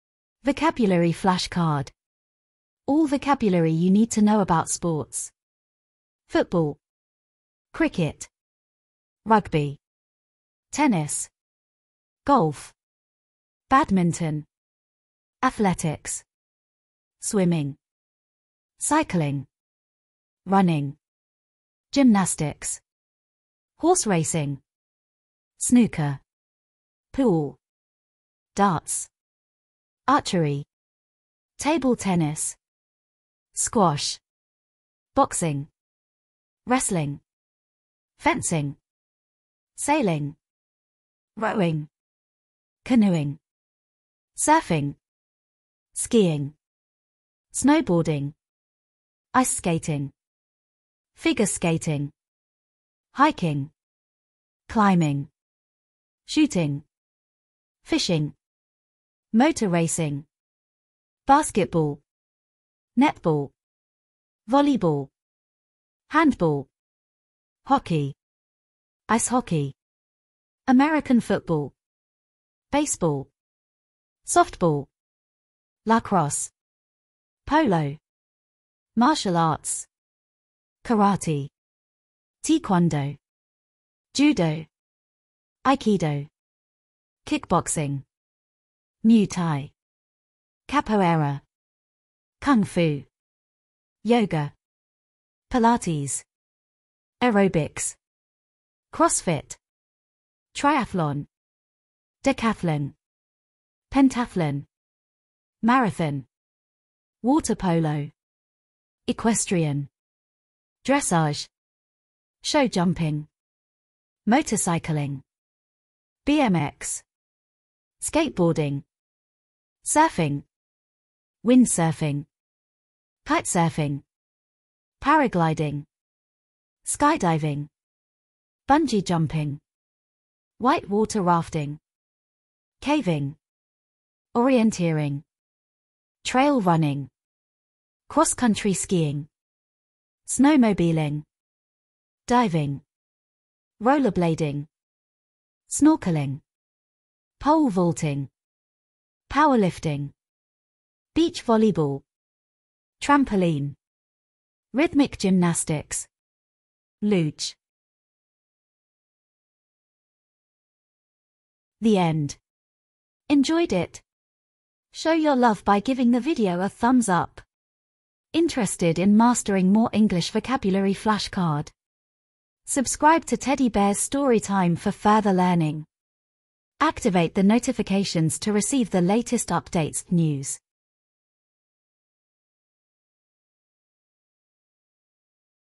Master All Sports Vocabulary Essential Words for Athletes & Enthusiasts Boost Your Sports Knowledge
Master All Sports Vocabulary: Essential Words and Phrases for Athletes and Enthusiasts | Boost Your Sports Knowledge!

🏀⚽🏈 Elevate your sports knowledge with our comprehensive guide - "Master All Sports Vocabulary: Essential Words and Phrases for Athletes and Enthusiasts"! 🌟📚 Immerse yourself in the dynamic language of sports, from terminology used in various games to expressions commonly heard in the athletic arena.

🎥 Video Highlights:

🗣️ Clear explanations of essential sports words and phrases.
📖 Comprehensive coverage for athletes and enthusiasts.
🎨 Visual aids to reinforce vocabulary retention.
🌈 Suitable for all levels, from beginners to sports aficionados.
👟 Perfect for:

🌐 Sports enthusiasts seeking to deepen their knowledge.
🏅 Athletes aiming to enhance their understanding of terminology.
📚 Language learners incorporating sports vocabulary into studies.

📖 Educational Benefits:
"Unlock the language of sports! This video is your gateway to mastering essential sports vocabulary, whether you're an athlete, an avid fan, or someone simply looking to broaden your language skills. Boost your sports knowledge and join us in this exciting linguistic journey!"

🌟🏀⚾ Let's enrich our lexicon and enhance our sports conversations together! 🚀📘✨